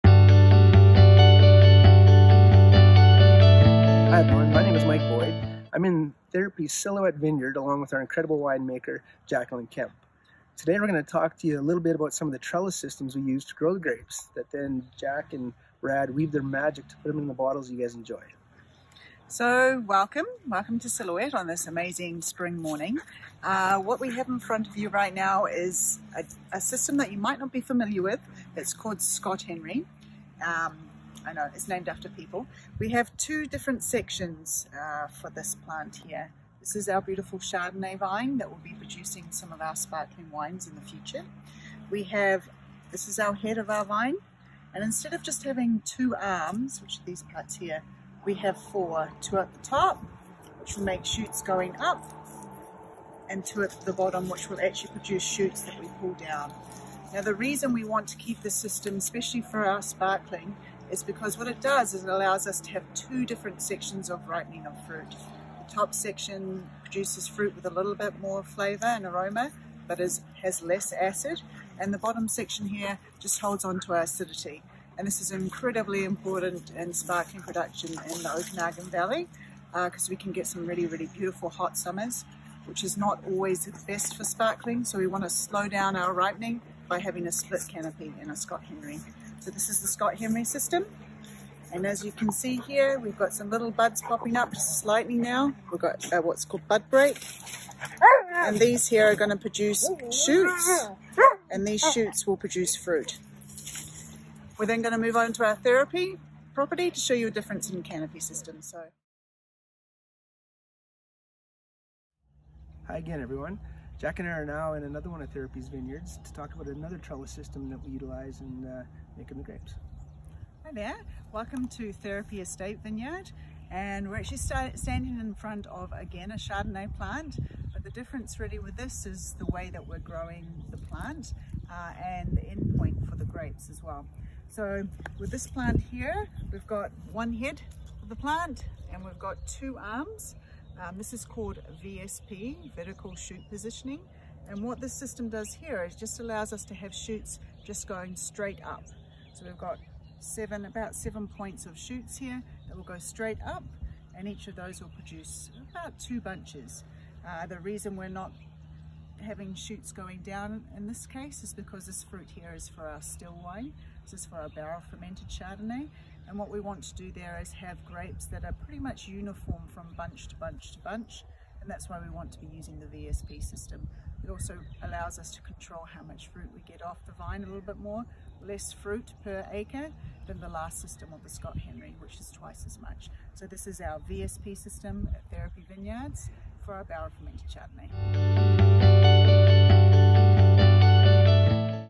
Vineyard Trellis Systems
Today we’re in the Therapy & home vineyards with our winemaker and viticulturalist, Jacq Kemp, to demonstrate and explain the how and why of the trellis systems in our vineyards. Bud break is a little slow this year but you can check out their progression since last week!